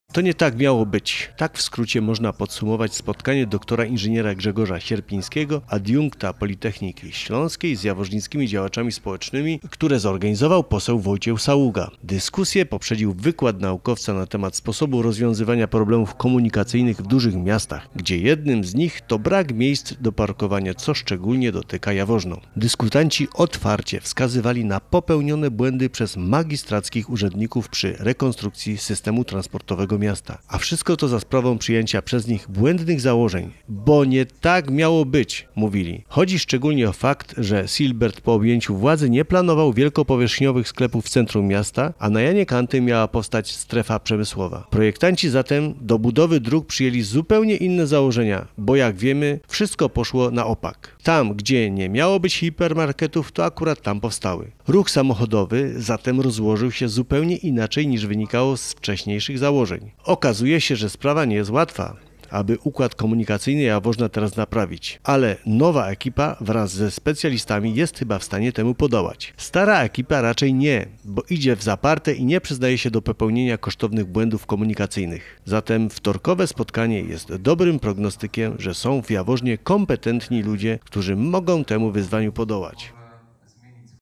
To nie tak miało być
Tak w skrócie można podsumować spotkanie dr. inż. Grzegorza Sierpińskiego, adiunkta Politechniki Śląskiej z jaworznickimi działaczami społecznymi, które zorganizował poseł Wojciech Saługa.
Dyskusję poprzedził wykład naukowca na temat sposobu rozwiązywania problemów komunikacyjnych w dużych miastach, gdzie jednym z nich to brak miejsc do parkowania, co szczególnie dotyka Jaworzno.
Dyskutanci otwarcie wskazywali na popełnione błędy przez magistrackich urzędników przy rekonstrukcji systemu transportowego miasta. A wszystko za sprawą przyjęcia przez nich błędnych założeń. Bo nie tak miało być -- mówili.
Chodzi szczególnie o fakt, że Silbert po objęciu władzy nie planował wielkopowierzchniowych sklepów w centrum miasta, a na Janie Kantym miała powstać strefa przemysłowa. Projektanci zatem, do budowy dróg przyjęli zupełnie inne założenia, bo jak wiemy wszystko poszło na opak. Tam, gdzie nie miało być hipermarketów, to akurat tam powstały. Ruch samochodowy zatem rozłożył się zupełnie inaczej niż wynikało z wcześniejszych założeń.
Okazuje się, że sprawa nie jest łatwa, aby układ komunikacyjny Jaworzna teraz naprawić. Ale nowa ekipa wraz ze specjalistami jest chyba w stanie temu podołać. Stara ekipa raczej nie, bo idzie w zaparte i nie przyznaje się do popełnienia kosztownych błędów komunikacyjnych.
Zatem wtorkowe spotkanie jest dobrym prognostykiem, że są w Jaworznie kompetentni ludzie, którzy mogę temu wyzwaniu podołać.